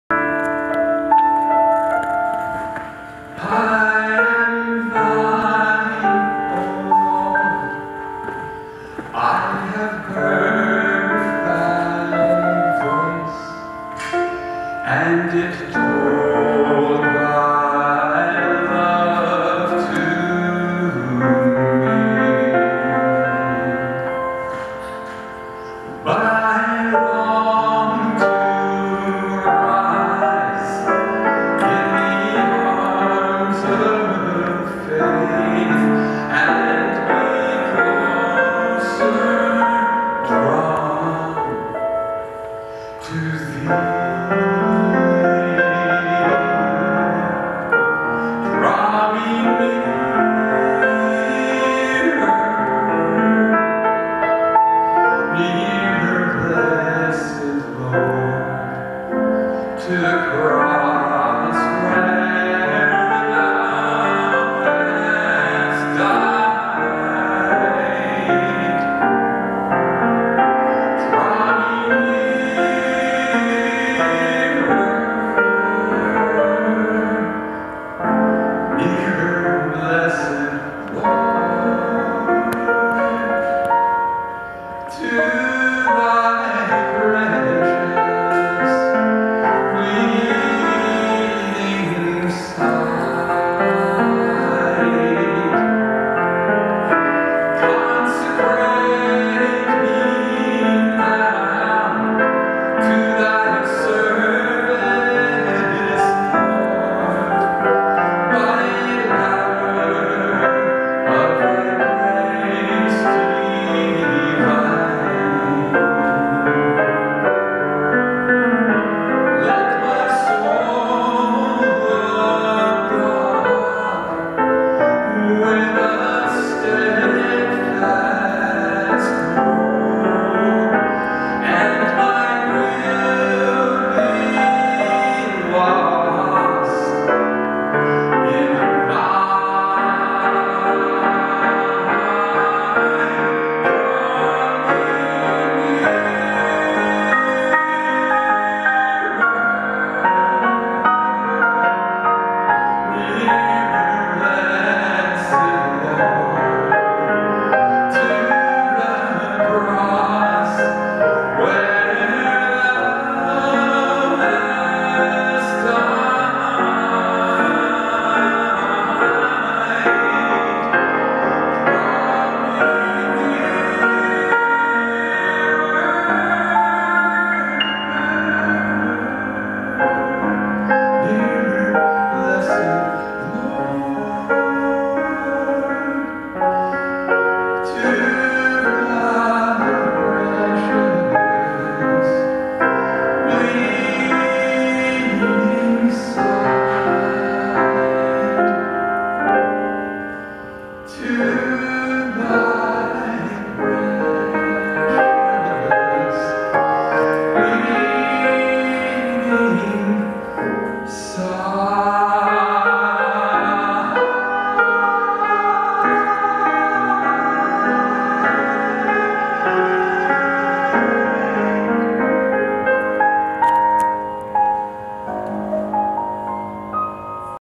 854A9642 Peter Scales
Peter Scales sings at Westminster Church Springfield, Illinois